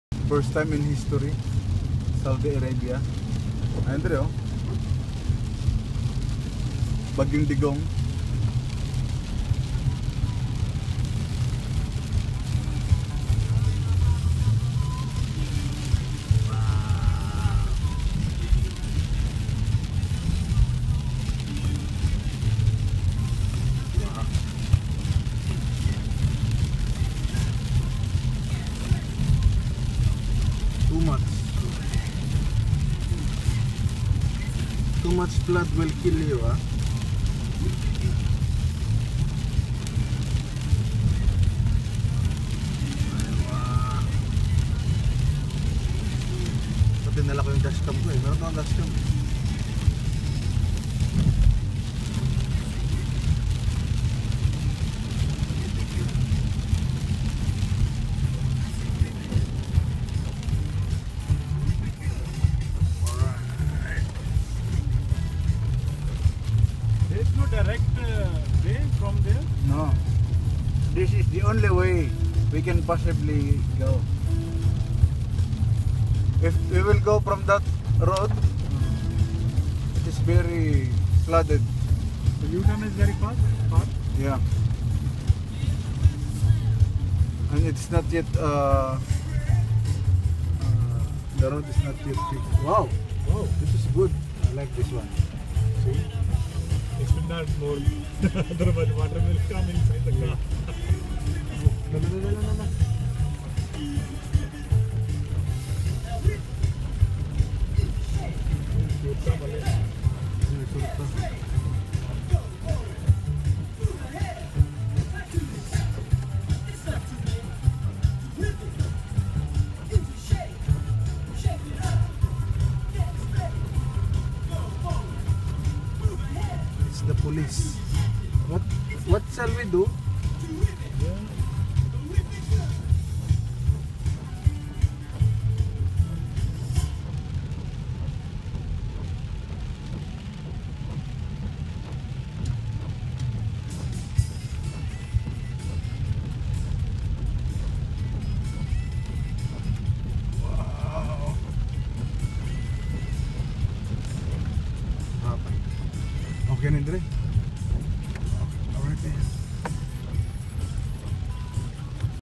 Dammam, AlDabab, KSA
Heavy pouring rain in SAUDI ARABIA.
Unusual weather condition.